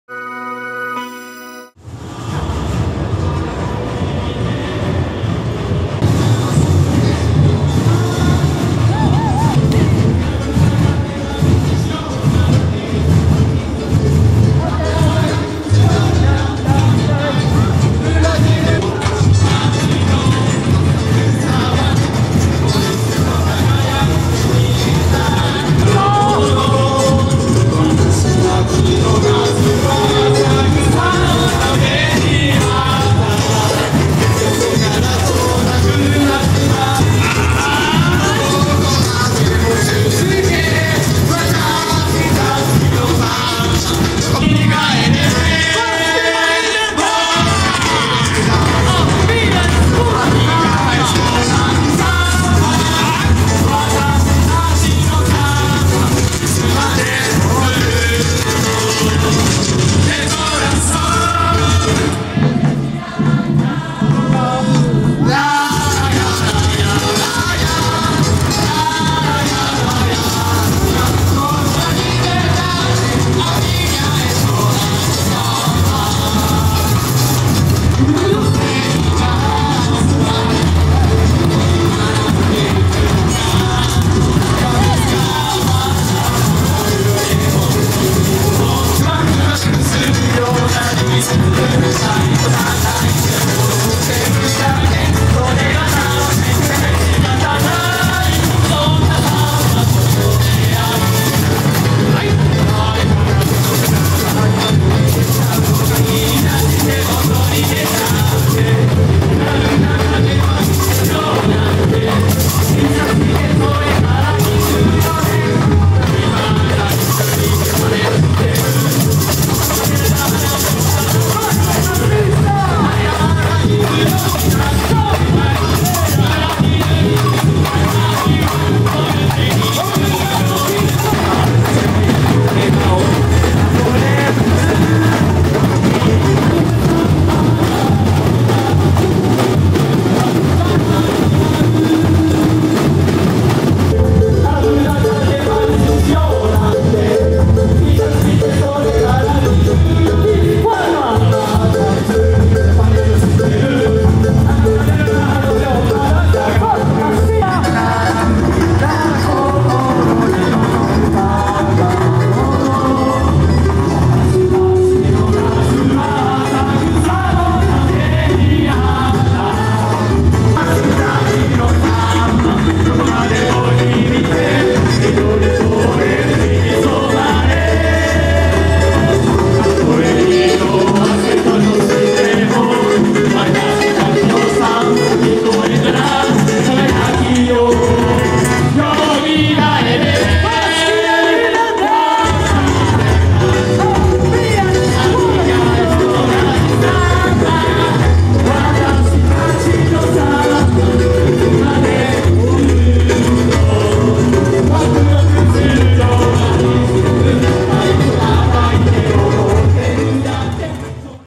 今までありがとう、ずっと忘れないよ！ 浅草サンバカーニバル2024 vol.6 自由の森学園サンバ音楽隊 ASAKUSA SAMBA CARNIVAL 2024
4K60P　2024年9月15日　今までありがとう、ずっと忘れないよ！
G.R.E.S BOSQUE DA LIBERDADE,Thank you for everything so far, I'll never forget you even after our activities end!
2016池袋ブラジル・ストリート・カーニバル　自由の森学園サンバ音楽隊　Festa do Brasil

浅草サンバカーニバル2024　vol.6　自由の森学園サンバ音楽隊、若さのパフォーマンス！　ASAKUSA SAMBA CARNIVAL 2024 G.R.E.S. BOSQUE DA LIBERDADE
5年振りに完全復活！の浅草サンバカーニバル　パレードコンテスト、今までありがとう、若さのパフォーマンス自由の森学園サンバ音楽隊、雷門手前です。
15 Sep. 2024